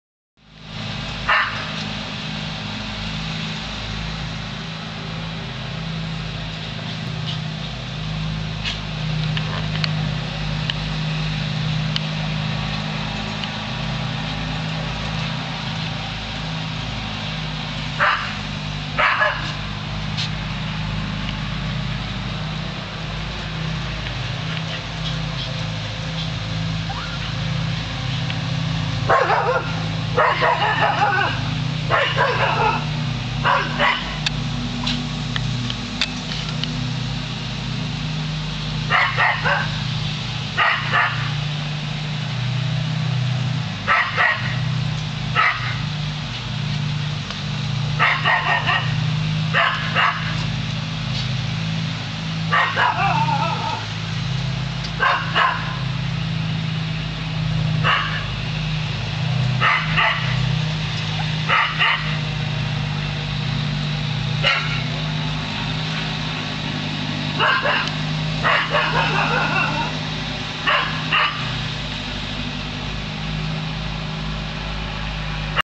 Intruder Alert!
Lasky, our border collie, barks at a hot air balloon.